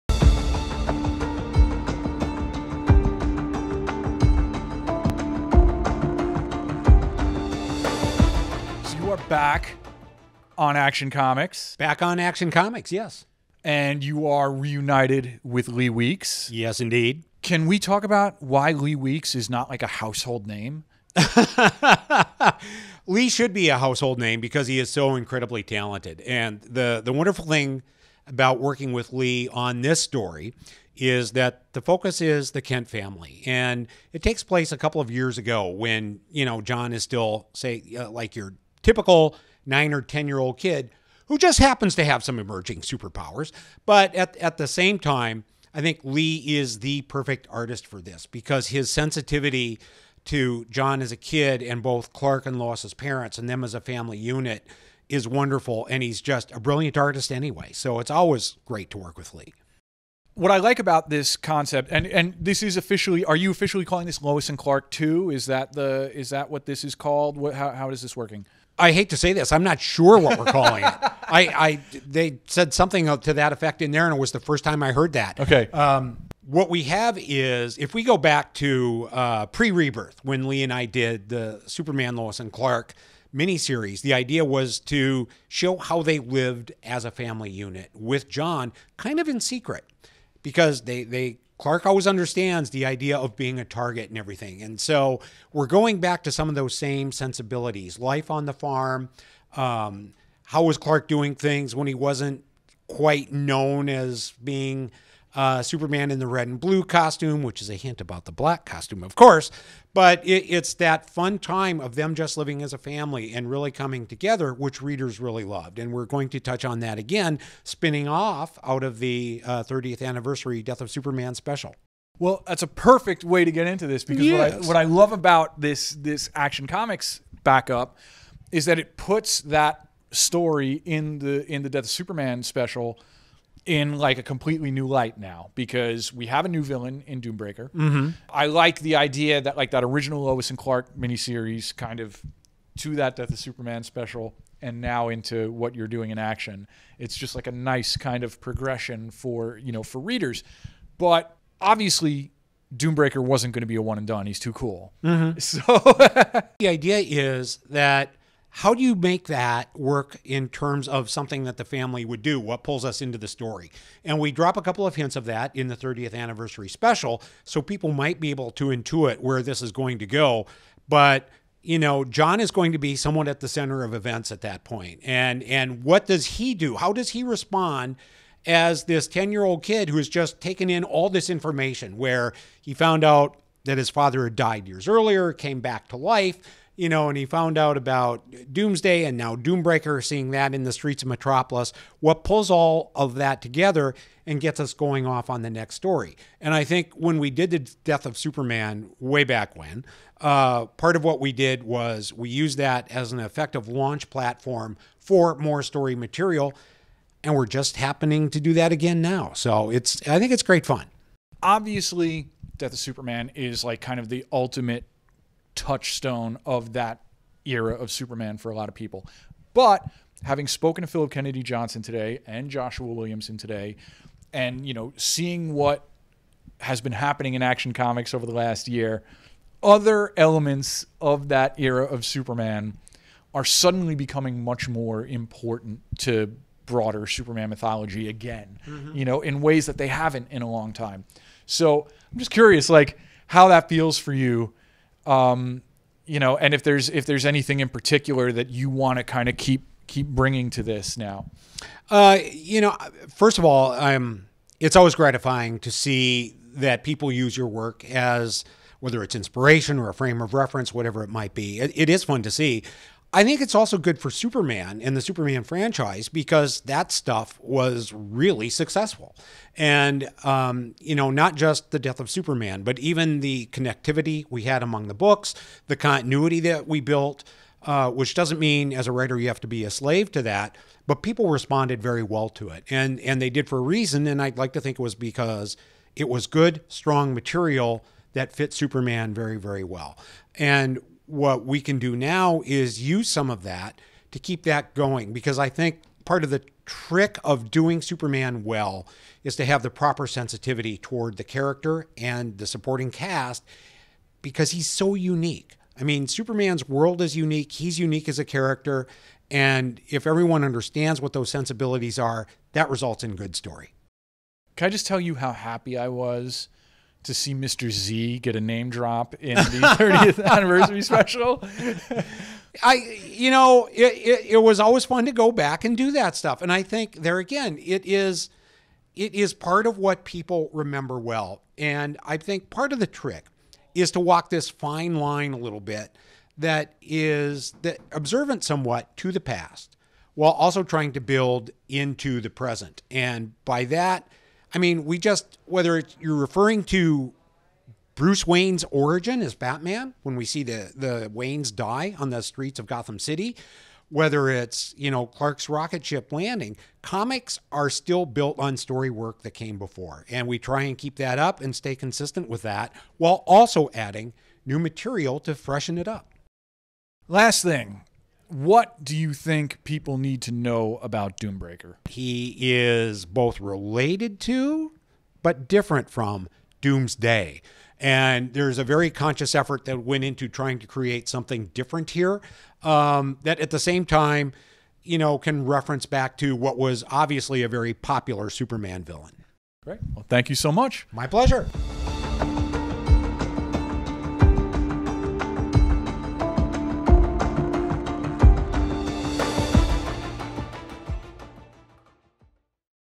SUPERMAN Legend Dan Jurgens Brings the Kent Family Back to the Farm | DC Standom
The legendary writer/artist of The Death of Superman, DAN JURGENS, brings the Kent family back to the farm with an exciting and heartfelt story about Jon Kent's younger days. In this special episode of DC Standom, Mike Cecchini speaks with Jurgens about how The Death of Superman 30th Anniversary Special impacted this story, and what fans might expect from these Jon Kent backup stories in upcoming issues of Action Comics.

Follow Den of Geek!